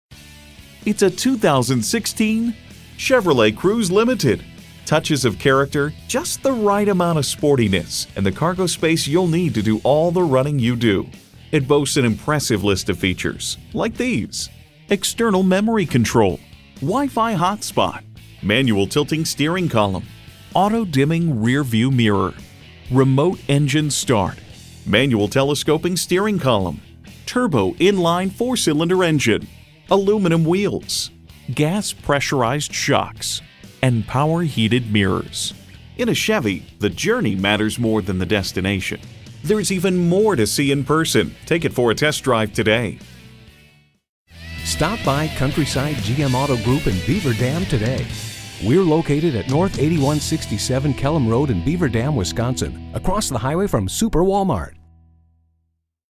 Used 2016 Chevrolet Cruze Limited Beaver Dam WI Madison, WI
Year: 2016
Make: Chevrolet
Model: Cruze Limited
Trim: 4dr Sdn Auto LT w/1LT
Engine: 1.4L I4 16V MPFI DOHC Turbo
Transmission: AUTOMATIC
Color: BLACK
Interior: Unspecified
Mileage: 34836
Stock #: UC1878
VIN: 1G1PE5SB6G7184737

Address:
8167 Kellom Rd